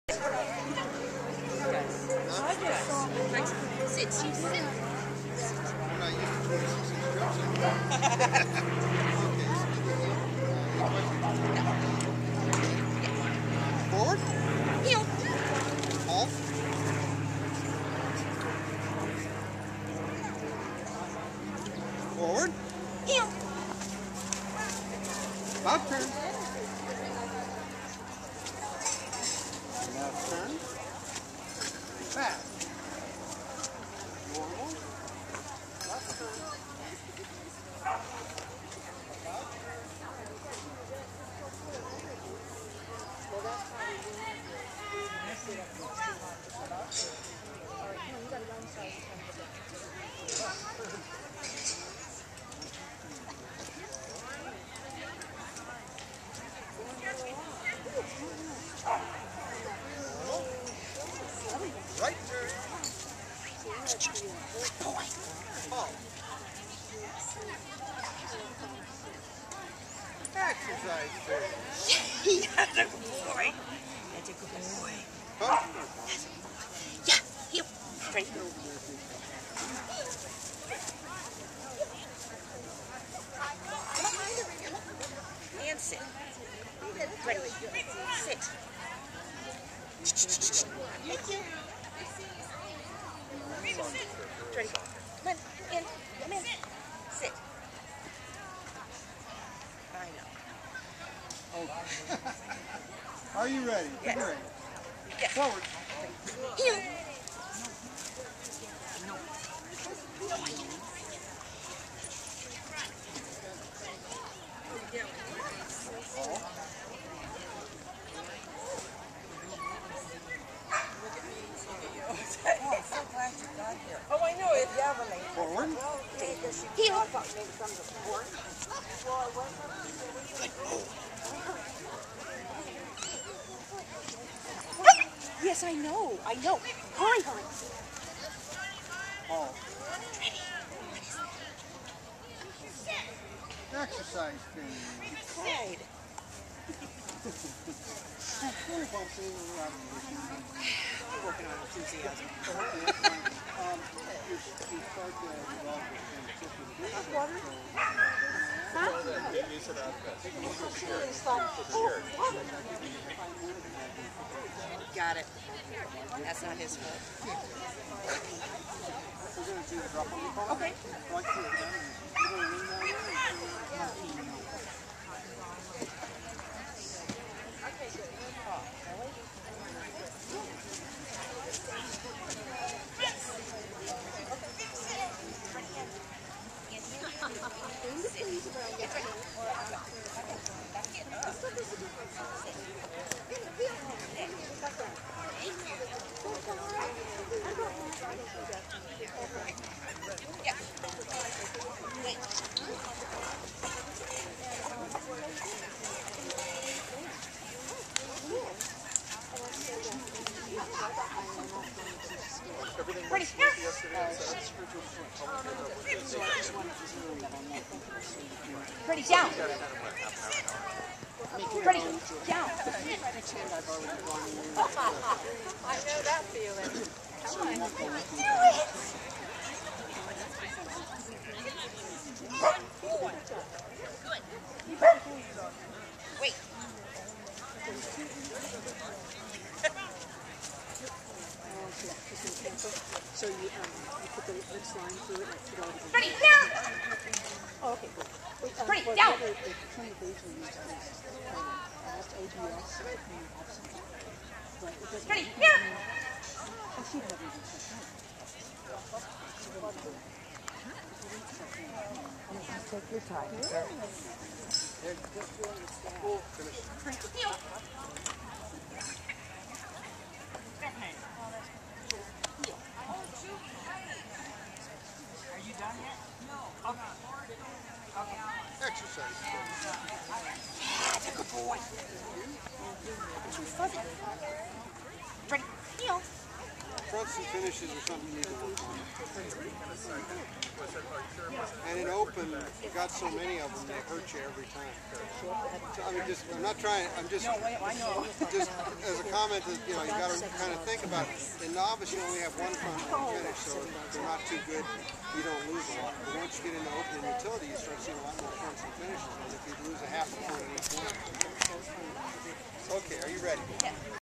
Andretti Open Obedience
Andretti officially Windrock Sin City Playboy CD RA makes his debute in Open Obedience at the match April 18th 2010 Handled by owner Lisa Stine